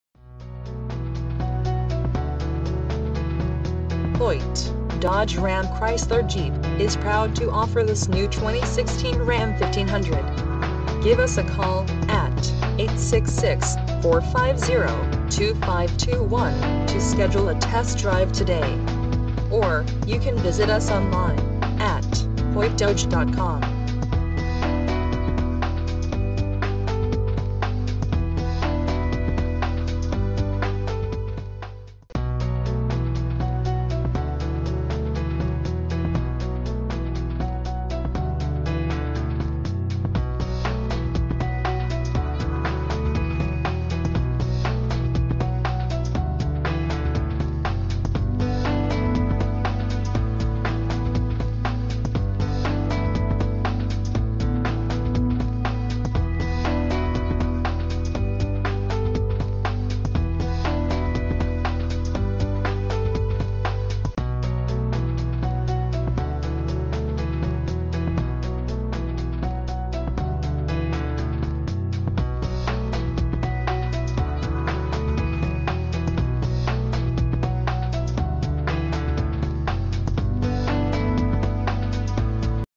2016 Dodge Ram 1500 Red 4X4 Crew Cab Lone Star Truck For Sale Pilot Point TX
2016 Dodge Ram 1500 Red 4X4 Crew Cab Lone Star Truck For Sale Pilot Point TX.
Hoyte Dodge RAM Chrysler Jeep is proud to offer Pilot Point TX this new 2016 Ram 1500 RED 4X4 CREW CAB LONE STAR.

2300 N. HWY 75